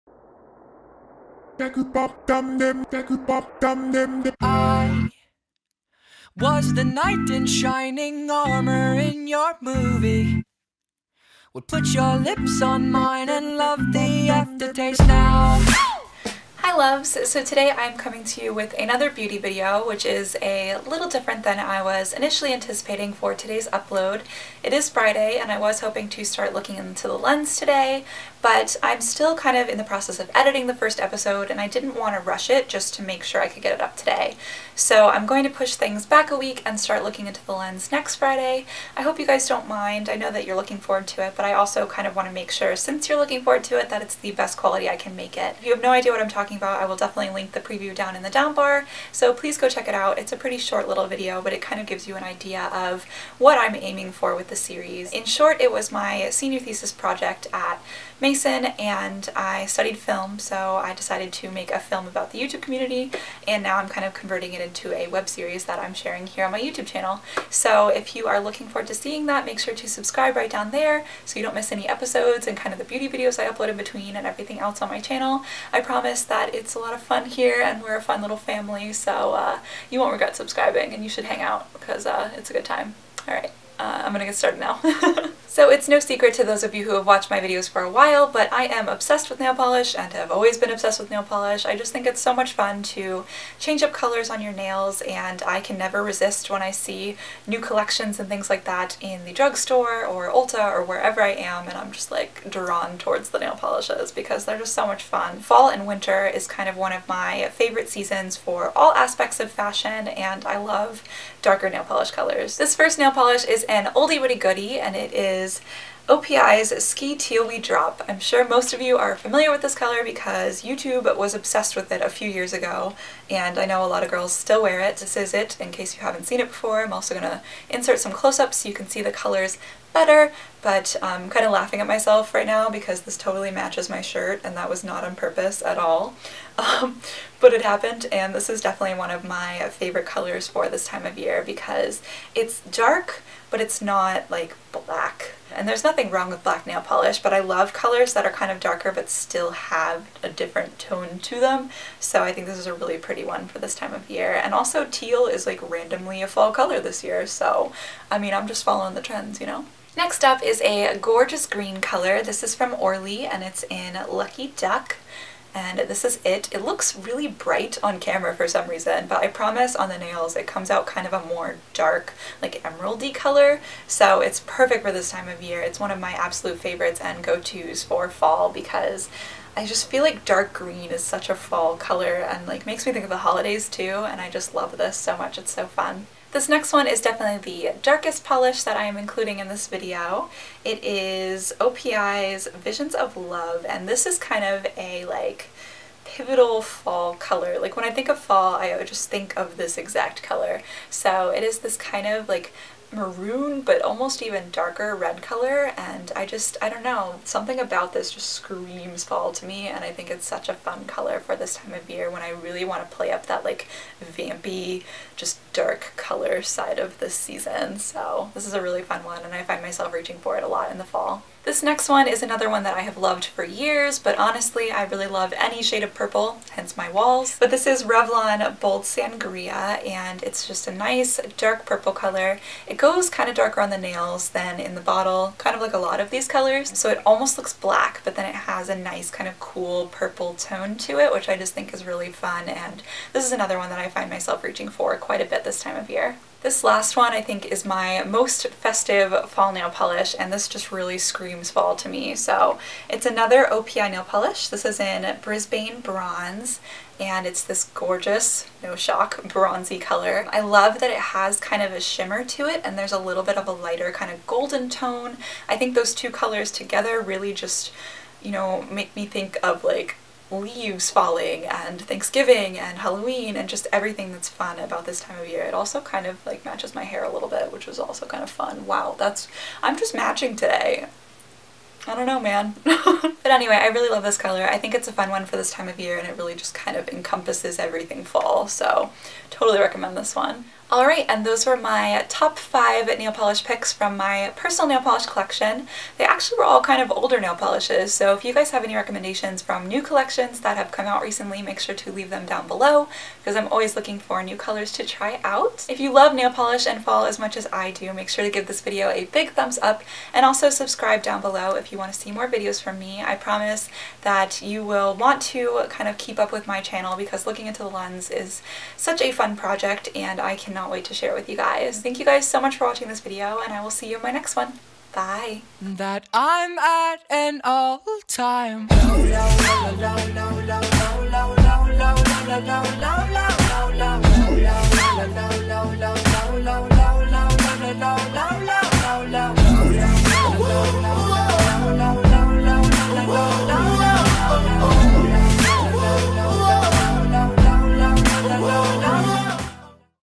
Top 5 Fall Nail Polishes | Curlyhairlove
Thumbs up for fall and being obsessed with nail polish ♥

Today I'm sharing some of my favorite nail polishes for fall with you guys! =)